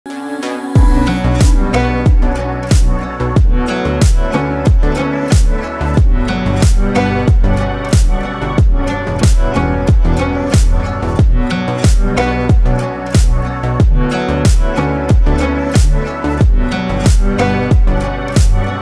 Would you spend Rs 6.24 crore on this convertible Ferrari?
Ferrari has launched the convertible 296 GTS in India at Rs 6.24 crore ex-showroom. This marks the arrival of both the 296 GTB and the GTS in the country. Here’s a short look at the Italian supercars.